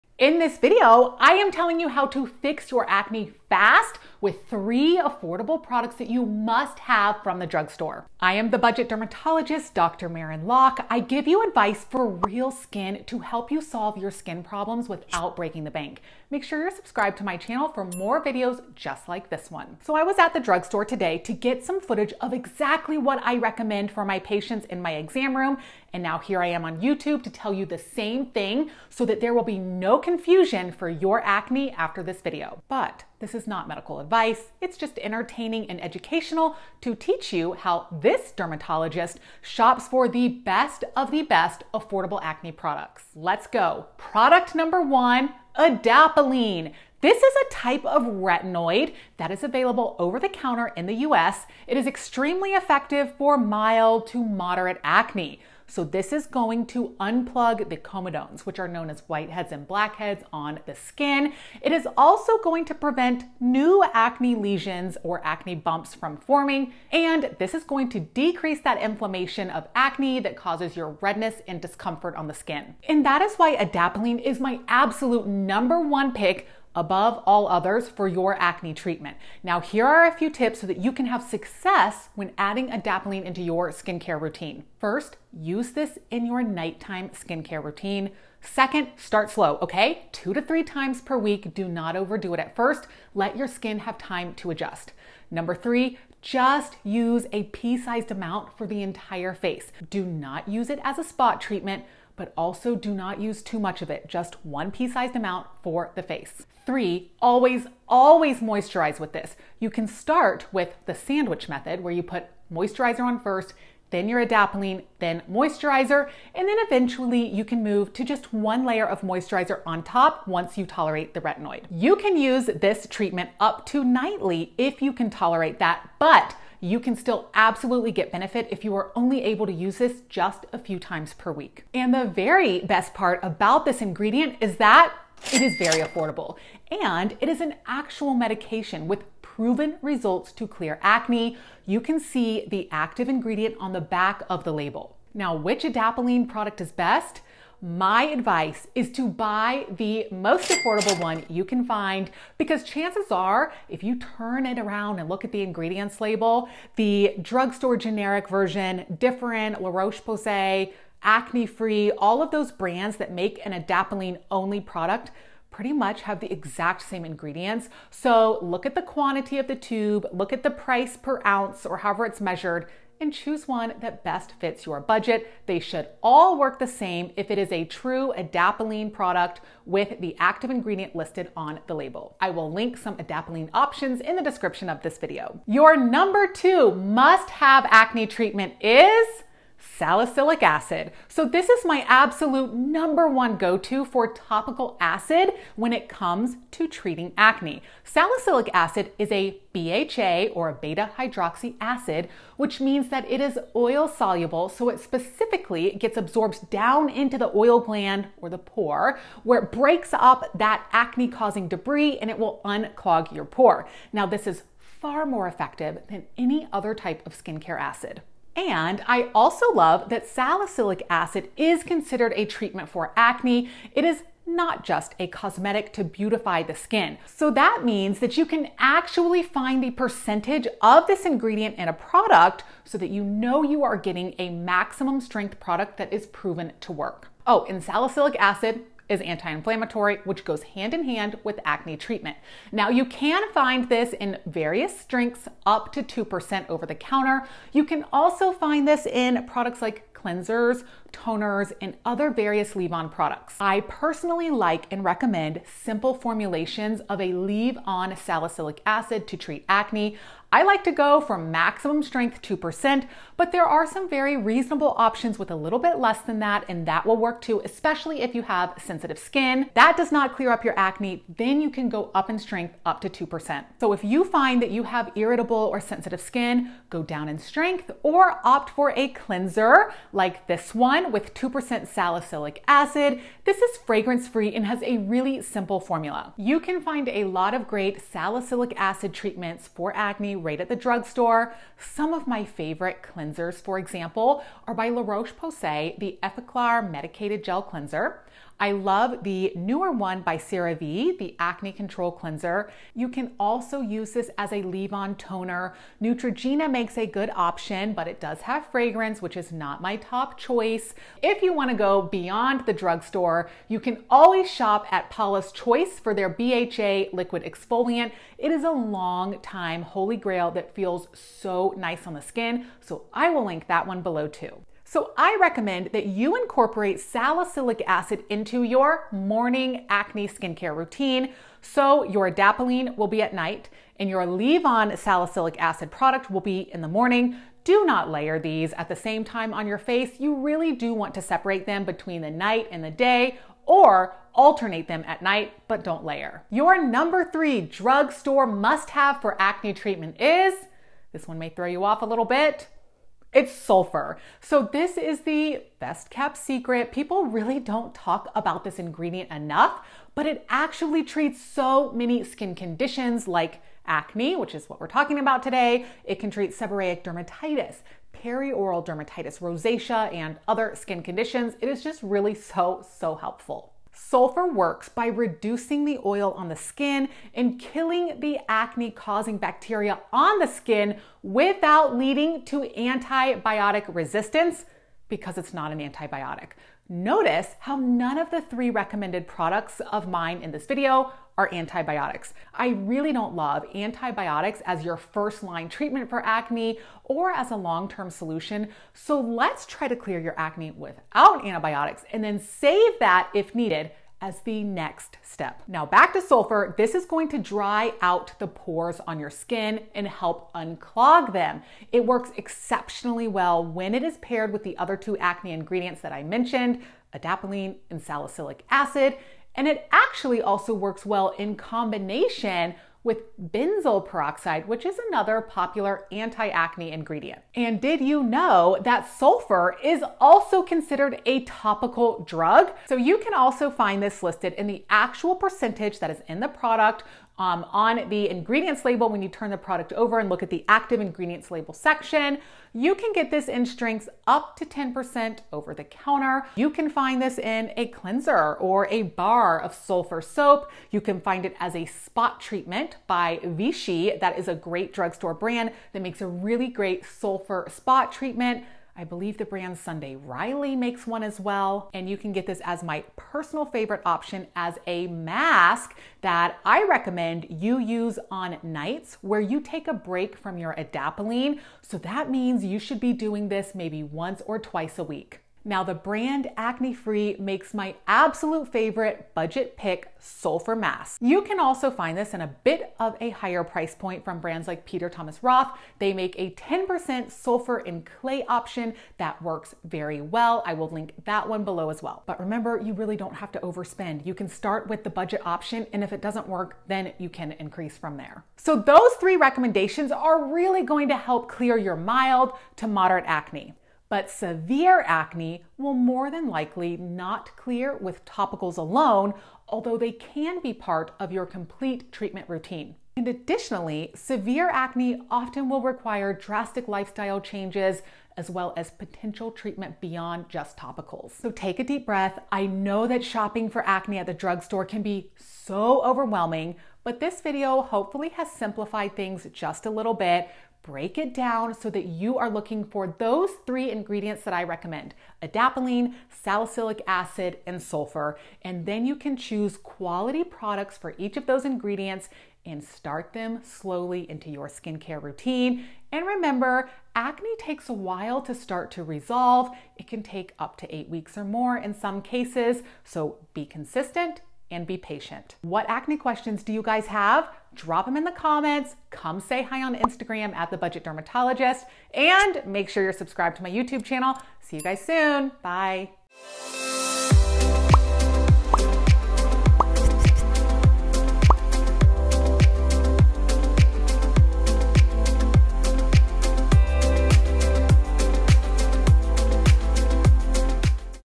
Shop With Me! 3 Acne Drugstore Must Haves | The Budget Dermatologist
These 3 Drugstore Acne Products are MUST-HAVES for your Acne skincare routine! Come shop with me! All products featured in this video are linked below :)

Products Mentioned In This Video :
Differin Gel Adapalene Gel 0.1%
Acne Free Adapalene Gel

Acne Free Sulfur Acne Foaming Cleanser
Sulfur Soap Grisi Acne Treatment Bar Soap

Fix Your Dark Spots in 3 Steps! | Hyperpigmentation | Melasma | Skincare Made Simple by The Budget Dermatologist